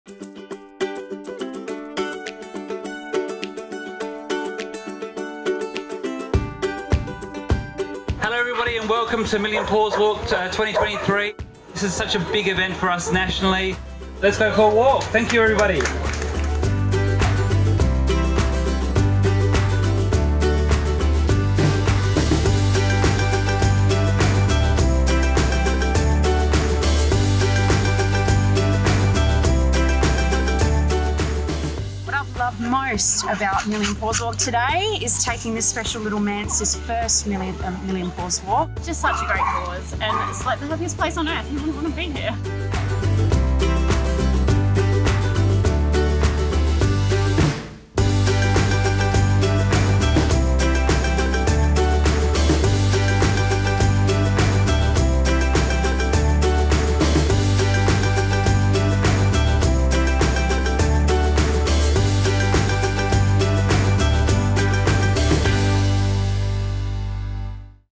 Million Paws Walk 2023
To everyone who braved the weather and joined us at the Adelaide Botanic Garden for our biggest (and best ever) Million Paws Walk, we simply cannot thank you enough 💙

It's the continued support from kind-hearted animal lovers like you that allows us to make the positive impact we do.

We also owe a big thank you to our friends at the Adelaide Botanic Garden, Royal Canin, Rufus & Coco Australia, Petbay, and Refuresh for making this special day possible 🐾

Visit our Facebook page to see all the amazing photos from the day 👇